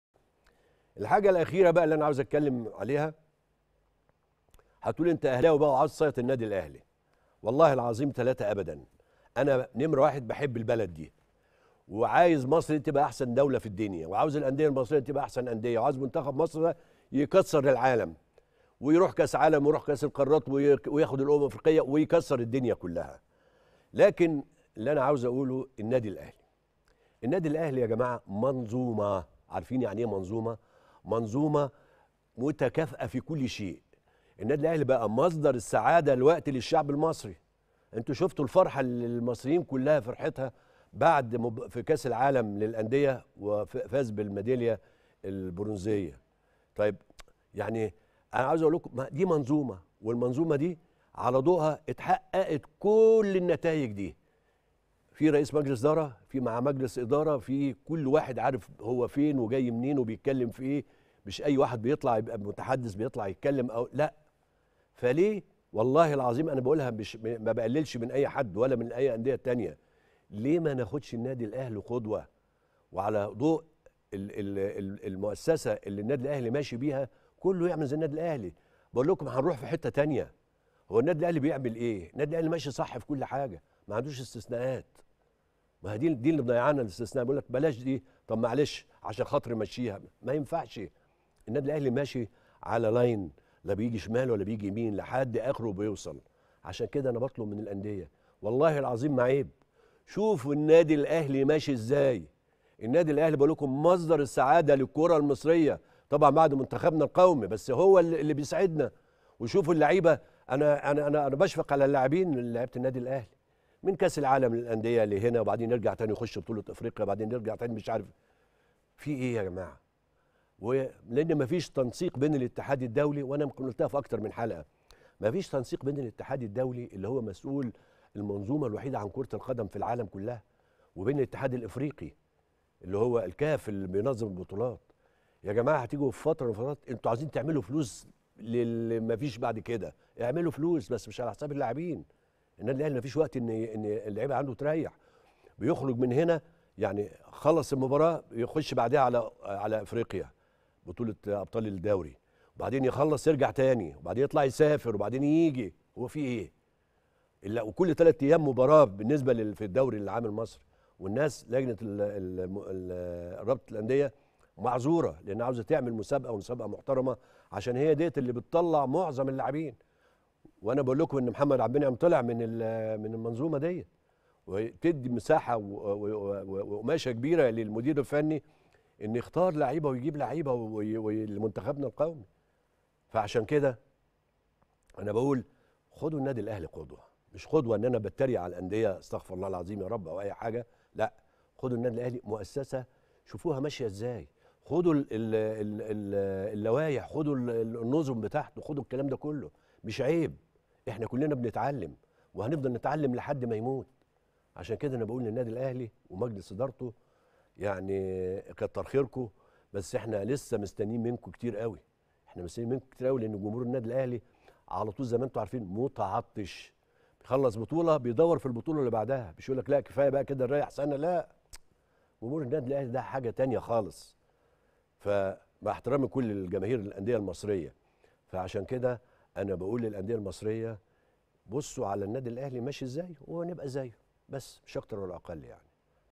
المجري: الأهلي منظومه متكاملة في كل شئ ومصدر السعادة للشعب المصري.. "مش عيب لما الأنديه تاخده قدوه"
المجري: النادي الأهلي منظومه متكاملة في كل شئ ومصدر السعادة للشعب المصري.. "مش عيب لما الأنديه تاخده قدوه"